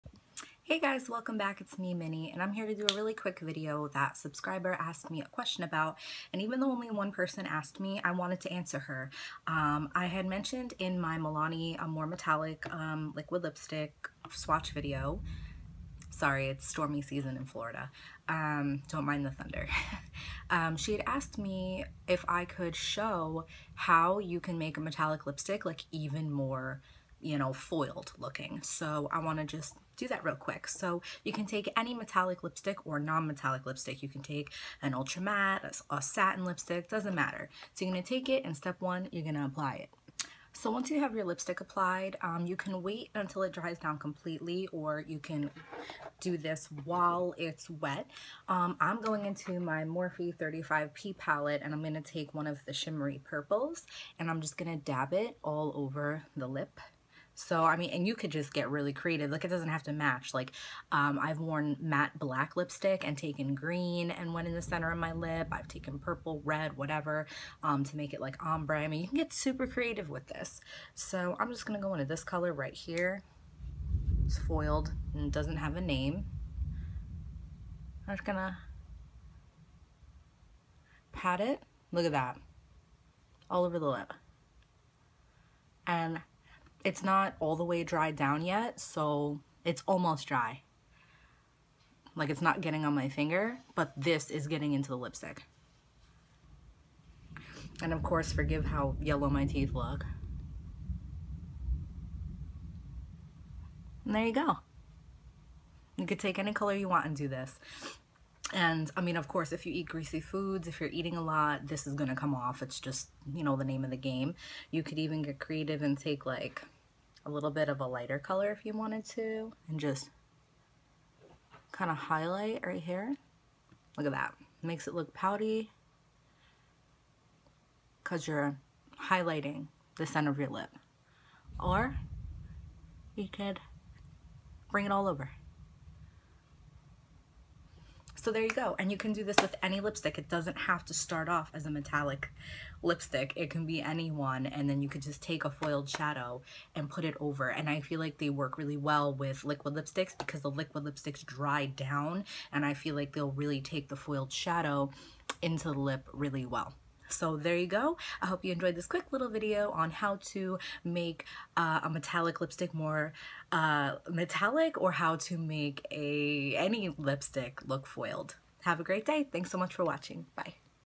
How To Create A Foiled Lip With Metallic Liquid Lipstick And Shimmery Shadow!
Hey sweeties! This was just a quick upload to answer the question of one of my amazing subscribers!

You can get super creative with this kind of look! Have fun! xoxo